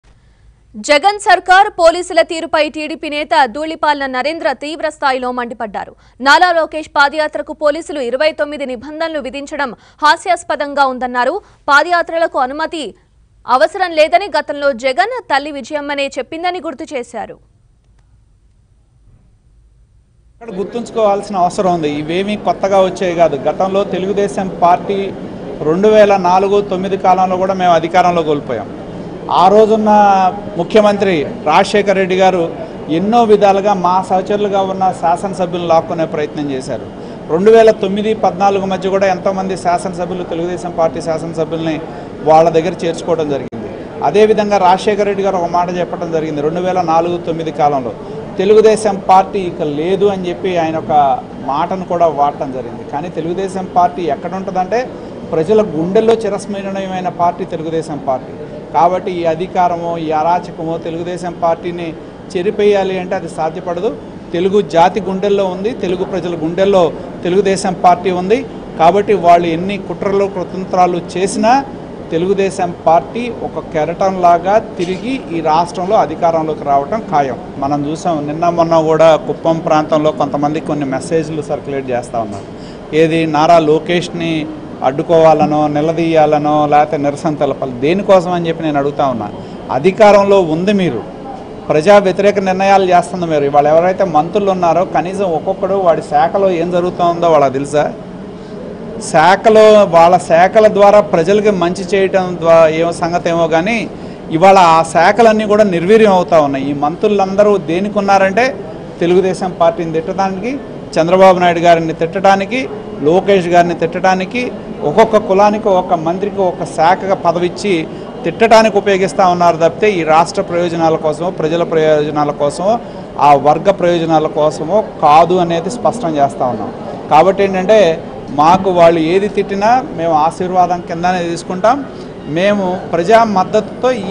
ఎందుకీ షరతులు..? | TDP Leader Dhulipalla Narendra Fires On YS Jagan Mohan Reddy | Lokesh Padayatra
నారా లోకేష్ పాదయాత్రకు నిబంధనల పై
జగన్ ప్రభుత్వం పై దూళిపాళ్ల నరేంద్ర ఆగ్రహం